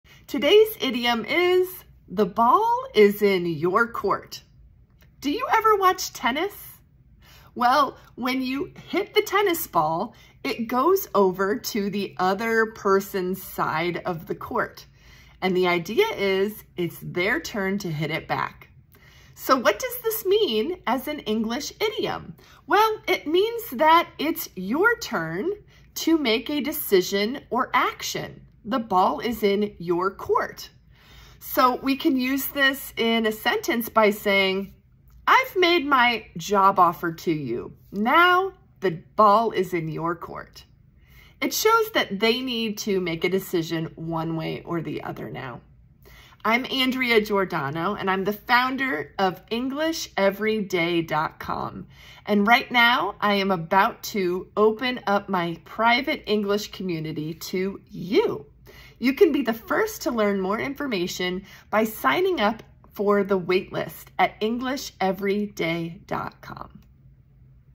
The ball is in your court
It's your turn to make a move! That's the idea behind the idiom **"the ball is in your court"**.  This means it's someone else's responsibility to take the next action or make a decision in a situation. Imagine a tennis match - after your opponent hits the ball, it lands on your side, so it's your turn to play.  For example, during a job interview, you might answer all their questions and then be told "the ball is in your court" - meaning they're waiting for you to decide if you're interested in moving forward.